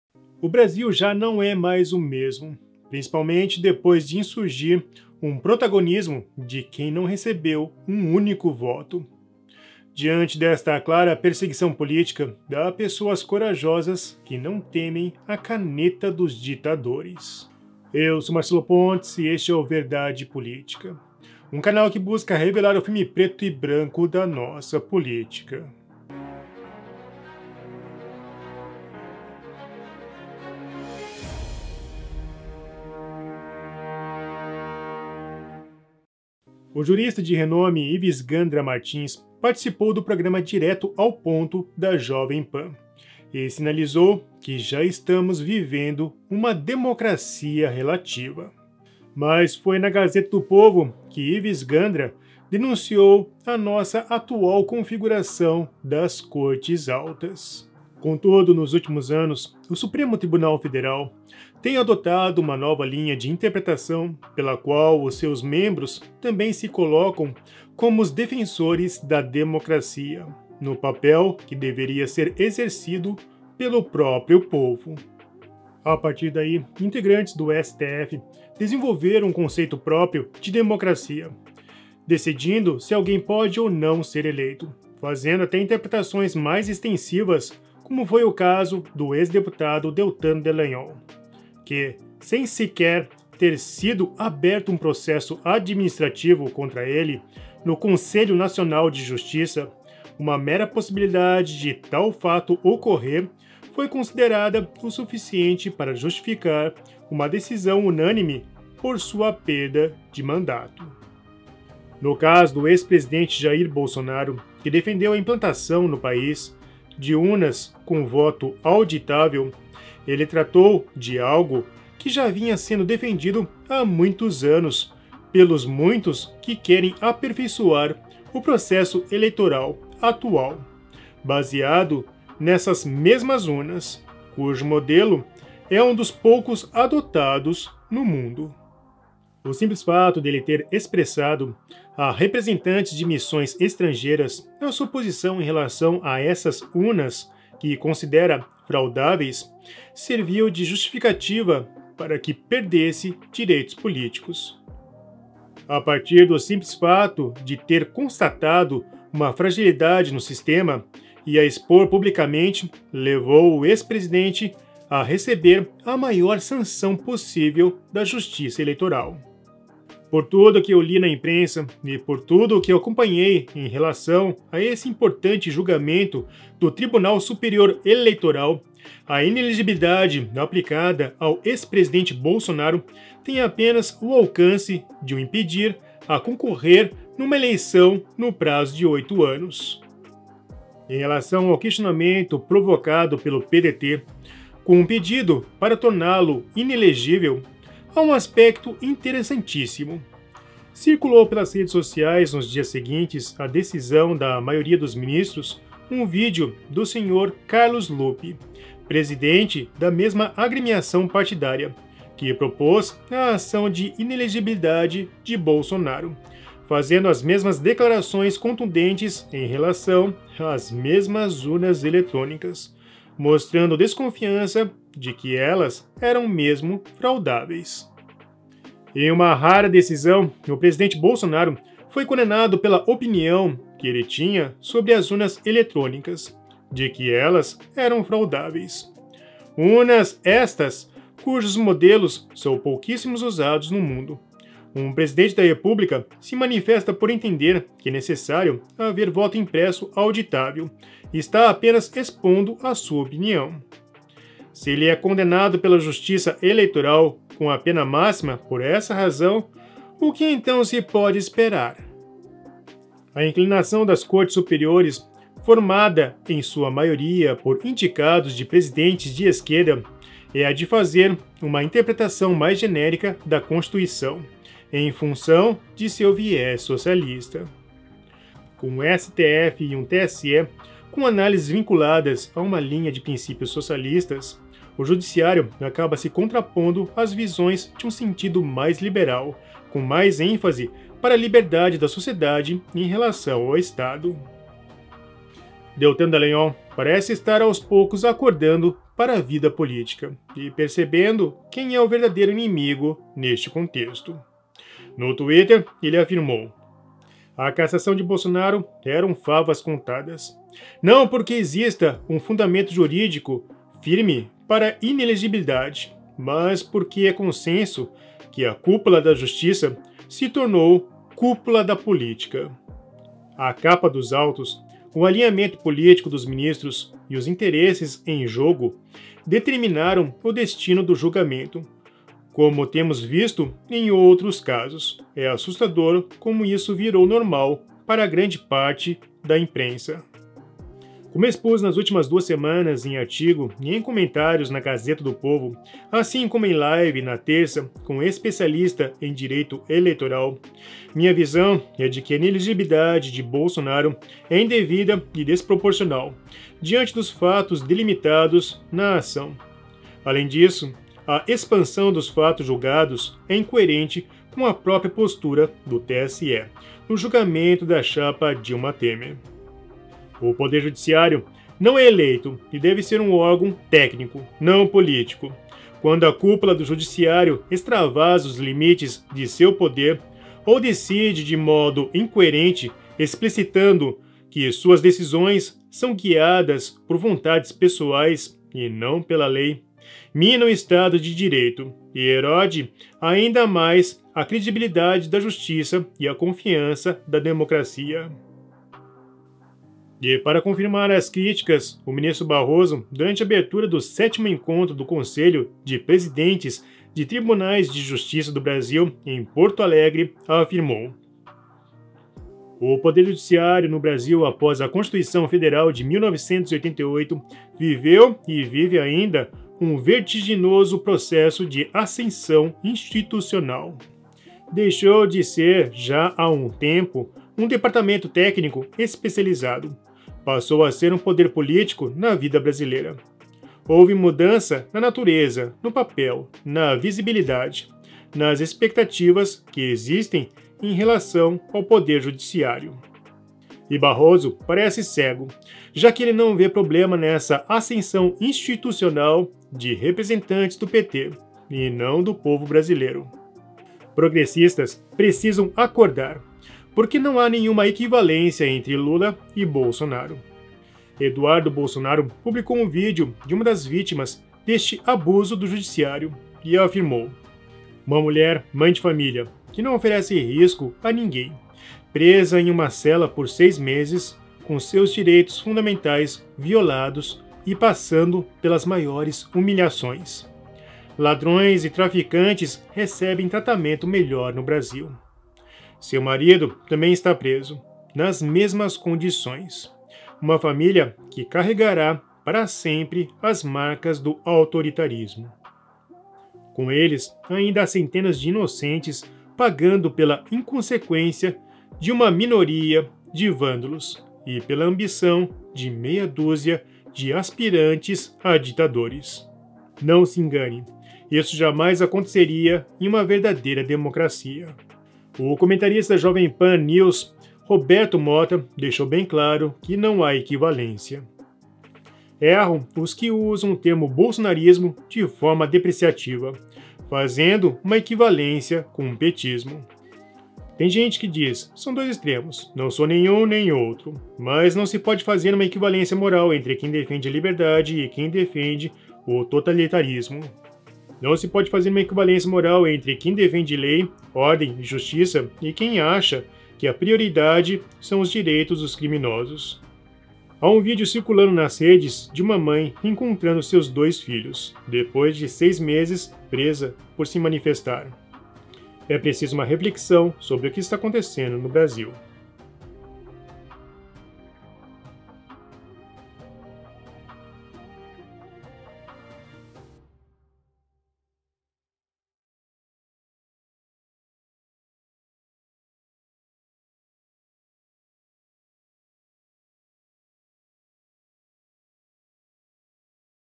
Ives Gandra REVELA toda a VERDADE
O Brasil não é o mesmo depois de insurgir um progranismo de quem não recebeu um único voto.

Diante desta clara perseguição política há pessoas corajosas que não temem a caneta dos ditadores.